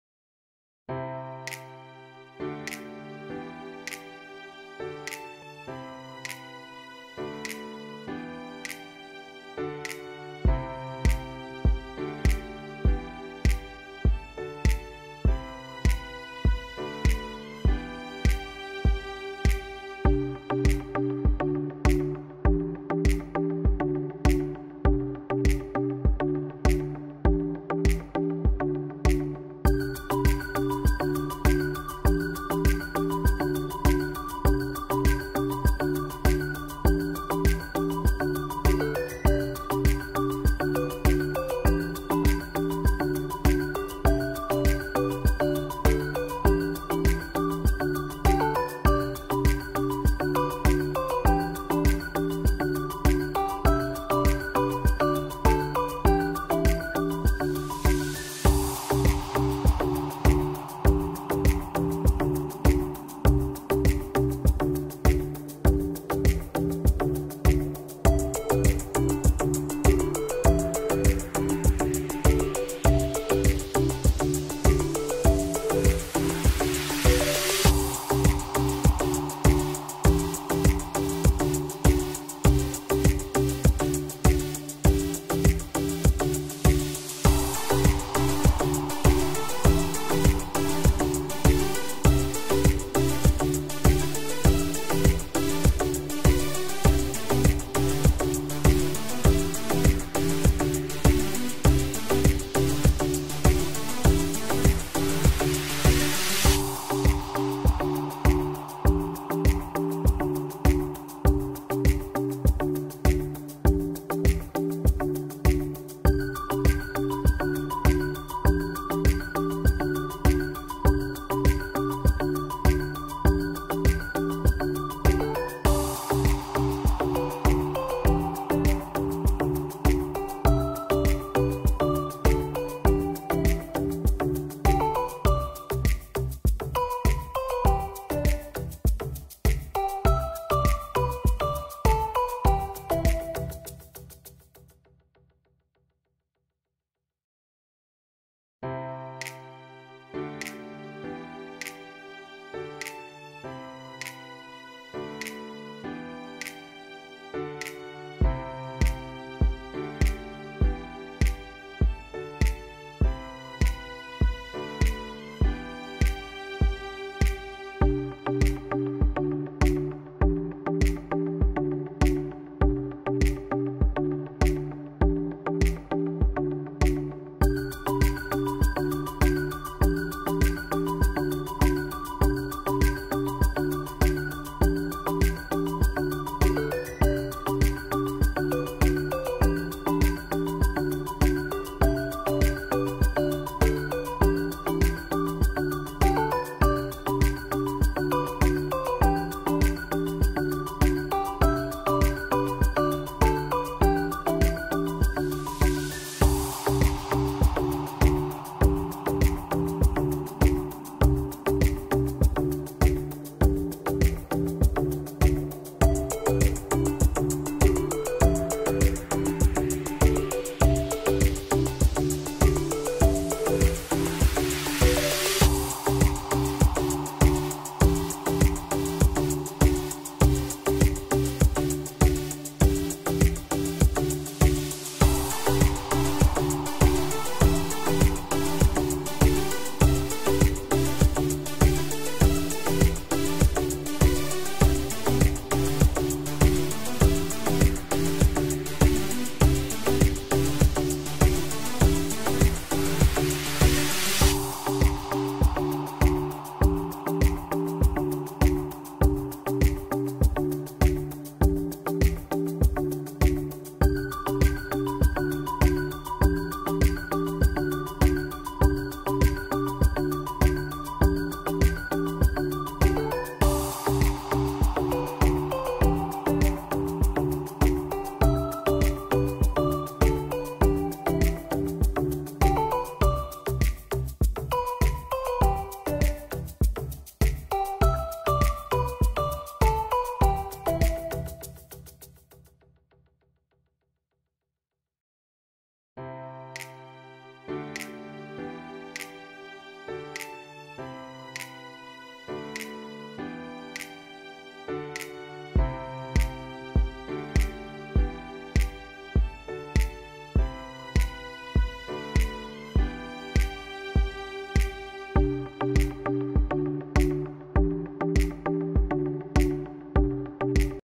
【ペーパークラフト】 Canon CreativePark 観覧車(レトロ)
ペーパークラフトの動く観覧車を作ります🎵